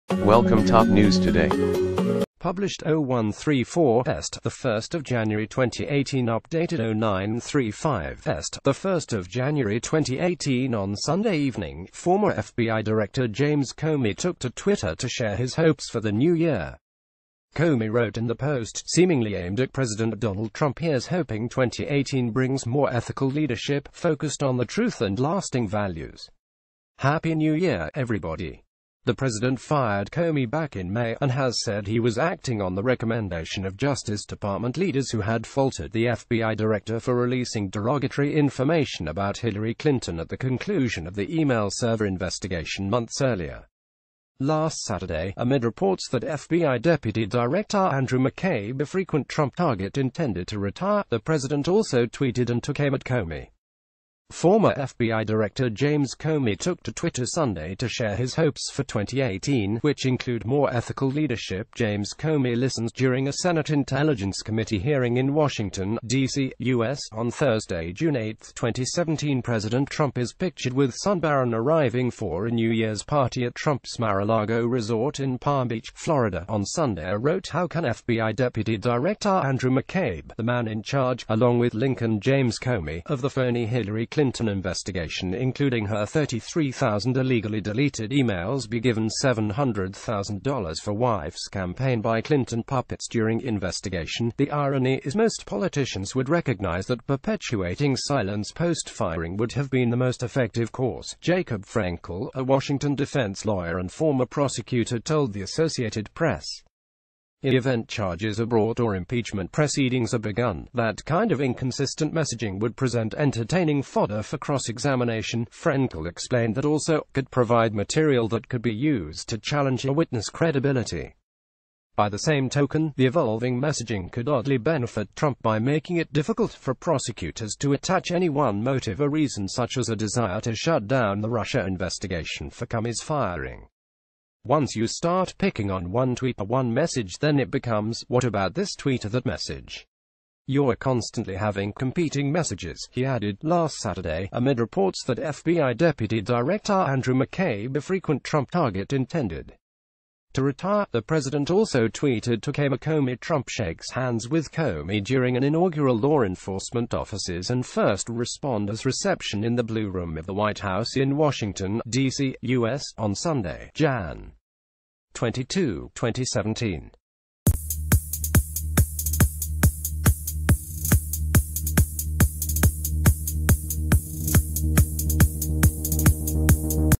BREAKING: James comey responded takes aim at trump on twitter
Published 0134 EST, 1 January 2018 Updated 0935 EST, 1 January 2018 On Sunday evening, former FBI director James Comey took to Twitter to share his hopes for the new year. Comey wrote in the post, see...